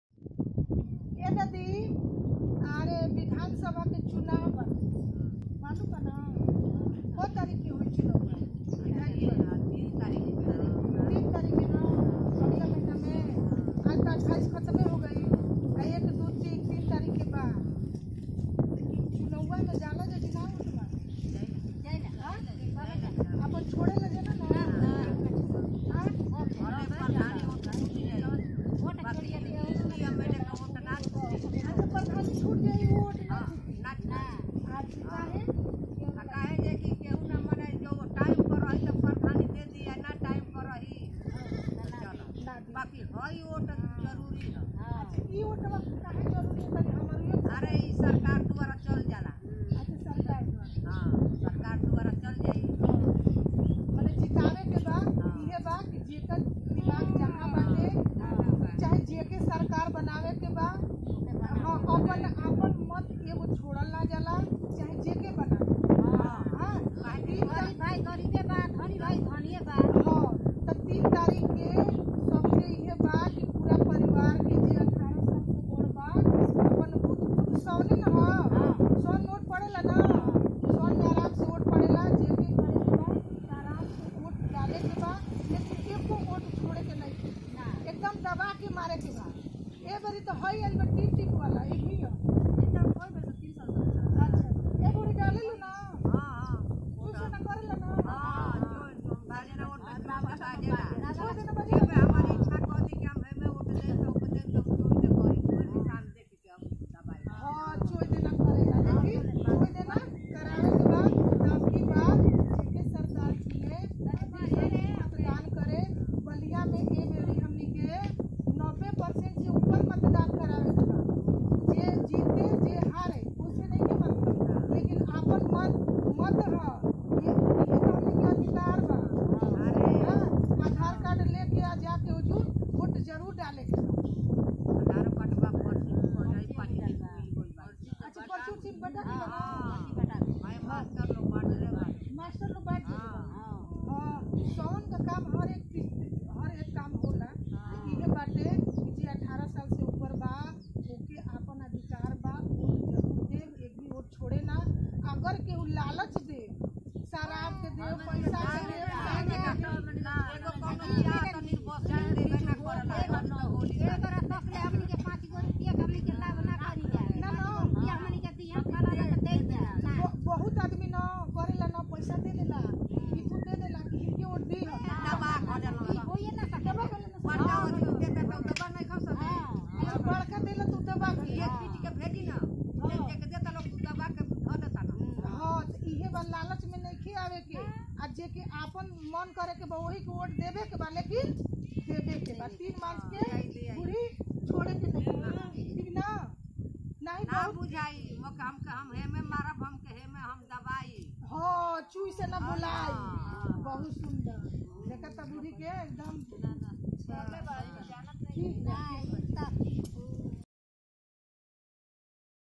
तीन मार्च को E.V.M का अवश्य बटन दबाये,अपनी मन योग्य एक अच्छी सरकार बनाये
मिशन शक्ति बलिया मतदाता जागरूकता अभियान पर जागरूक करते हुवे